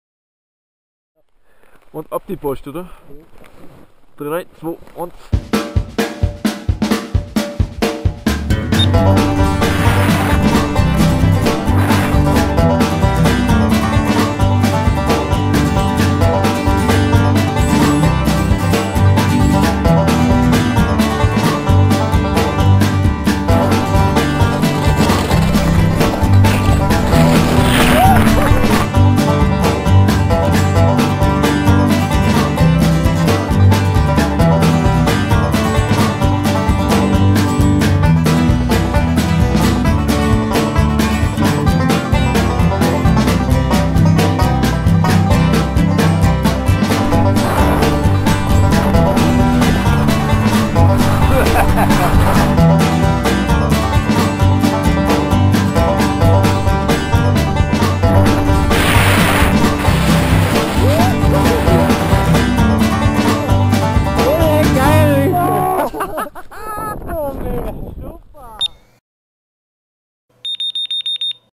Behind the Scene in Russia - Whiteroom & Rollei Actioncam
A short Behind the Scene Edit from our trip to Altai, Russia in May 2014.